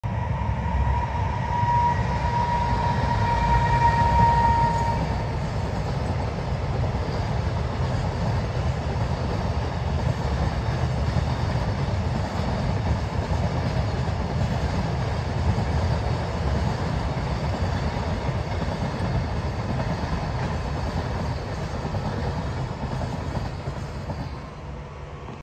北陸線貨物列車
福井県今庄
柿の木が多く見られました
吊るし柿が名産の様です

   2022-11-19